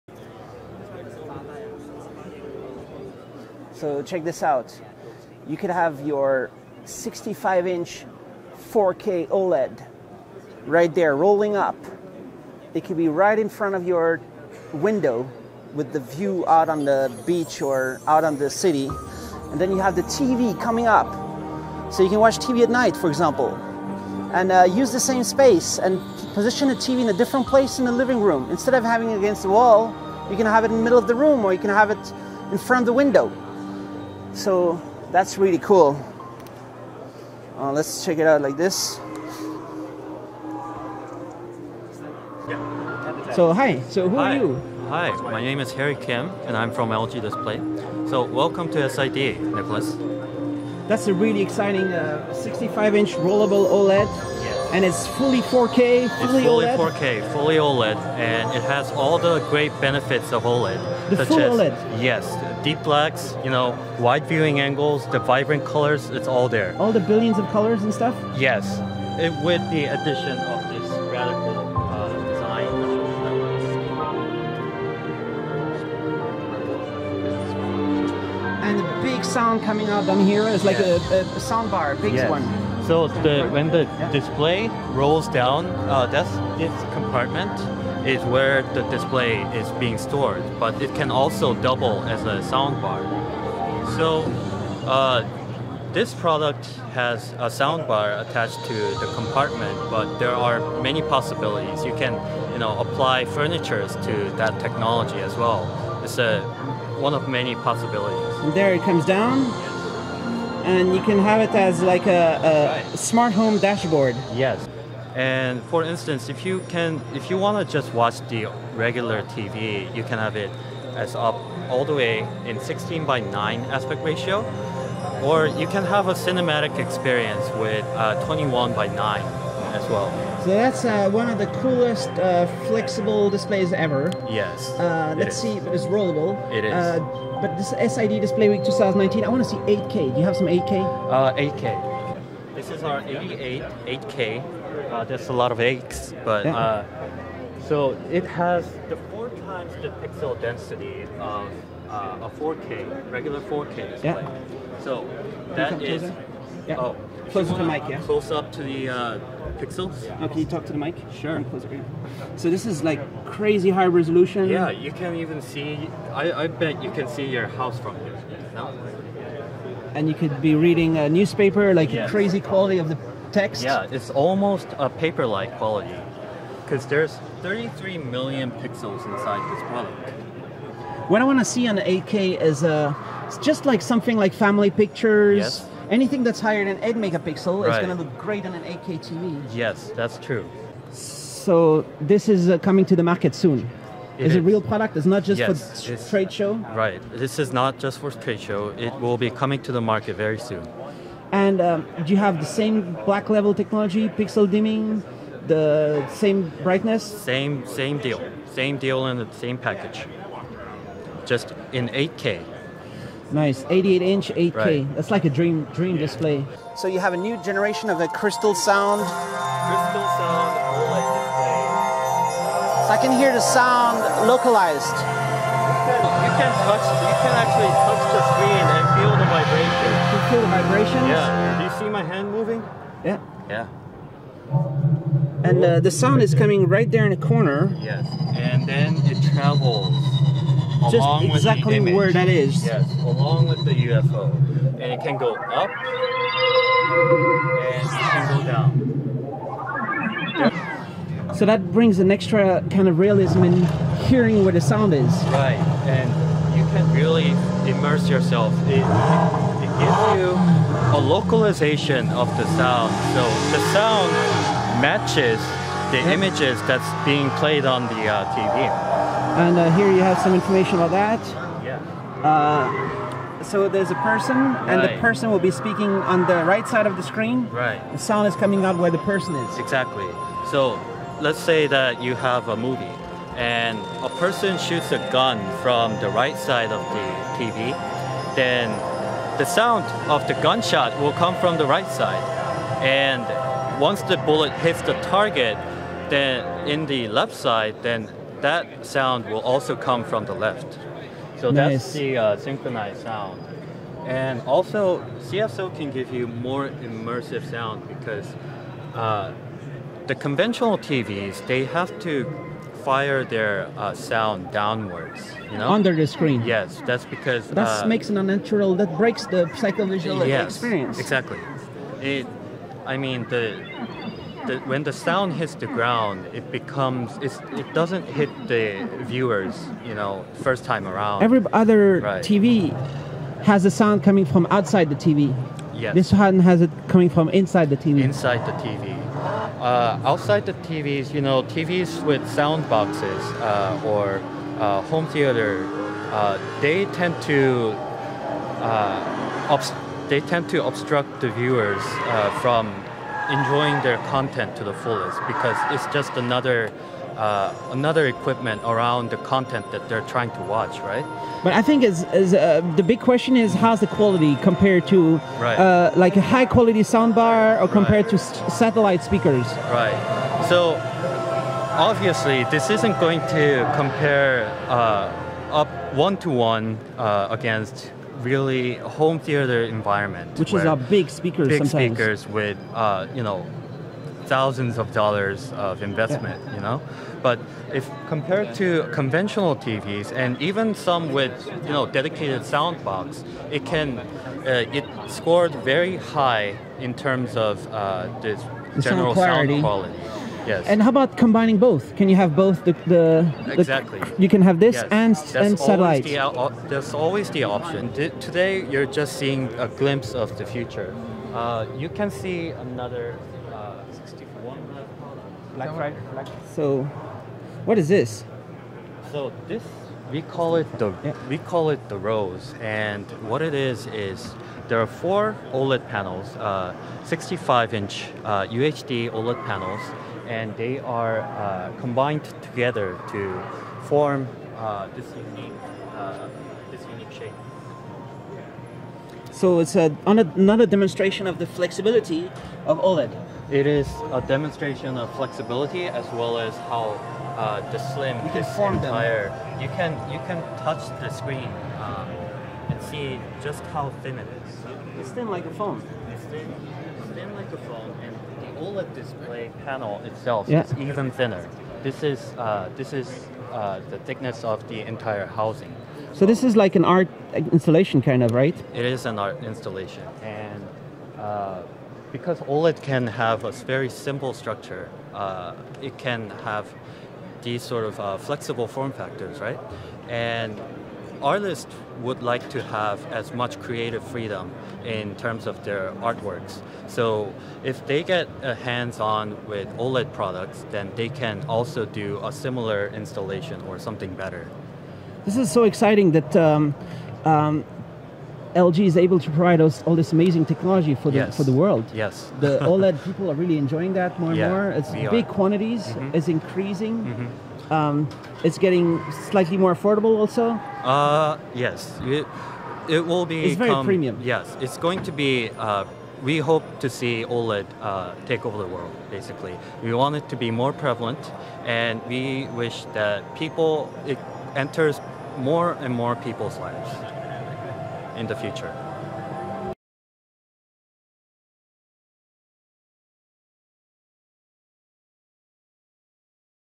LG Display 65'' Rollable OLED, 88'' 8K OLED, 8K Crystal Sound OLED, The Rose
LG Display shows latest OLED displays at SID DisplayWeek 2019, this booth features the new 65" Rollable OLED that can fit inside the sound bar or furniture and be positionned in the middle of the room or in front of the window in the living room, the 88" 8K OLED display, the latest 8K Crystal Sound OLED, which implements exciters to the super-slim OLED panel to generate localized crystal-clear sound directly from the display itself and The Rose featuring 4 65" 4K OLED. Thanks to Conan for filming the second camera angle edited into this video filmed on Panasonic G85 on the DJI Ronin gimbal stabilizer. You can find out more information about OLED an online community for OLED. Thanks to Conan for filming and providing the second camera angle giving an unprecedented view of how a Charbax Video is made. Here edited into this video, Conan filmed his camera angle using the Panasonic G85 on the DJI Ronin gimbal stabilizer.

Filmed in 4K60 at SID Display Week 2019 with Panasonic G9 ($1131